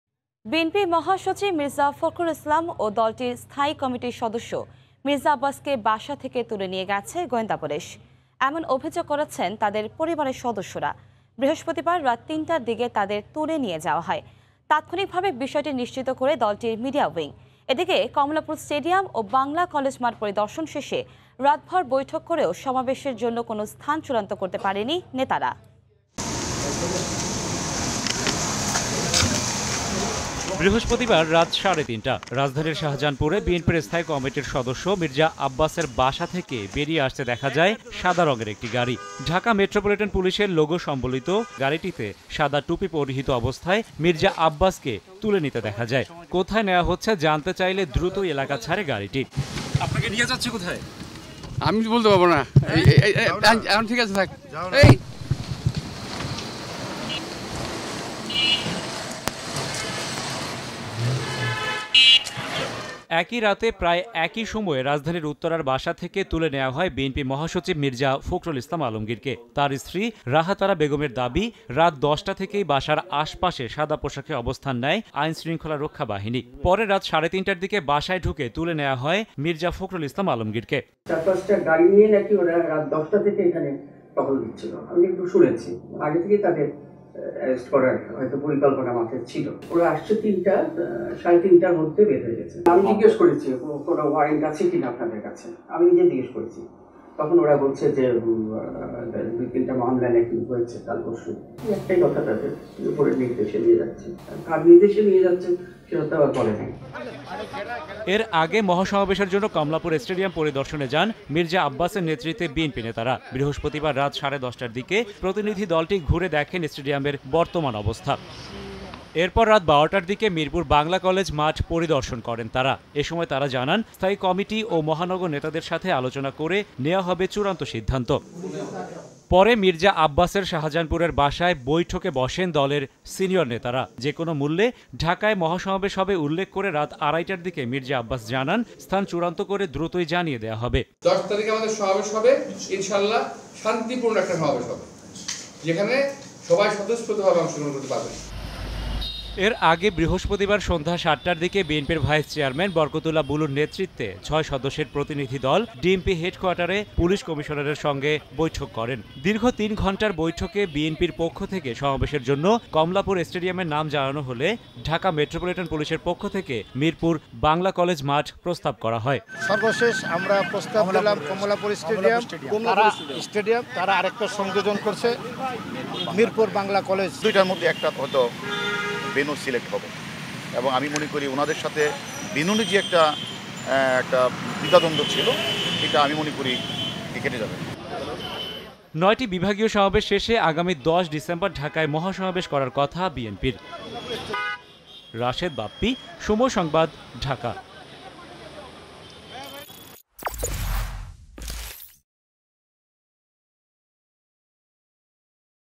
গভীর রাতে যেভাবে তুলে নেয়া হলো ফখরুল-আব্বাসকে | BNP Leaders | Mirza Fakhrul | Mirza Abbas | Somoy TV
বিএনপি মহসচিব মির্জা ফখরুল ইসলাম ও দলটির স্থায়ী কমিটির সদস্য মির্জা আব্বাসকে বাসা থেকে তুলে নিয়ে গেছে গোয়েন্দা পুলিশ। এমন অভিযোগ করেছেন তাদের পরিবারের সদস্যরা। বৃহস্পতিবার রাত তিনটার দিকে তাদের তুলে নিয়ে যাওয়া হয়। তাৎক্ষণিকভাবে বিষয়টি নিশ্চিত করে দলটির মিডিয়া উইং। এদিকে, কমলাপুর স্টেডিয়াম ও বাংলা কলেজ মাঠ পরিদর্শন শেষে, রাতভর বৈঠক করেও সমাবেশের জন্য কোনো স্থান চূড়ান্ত করতে পারেননি নেতারা।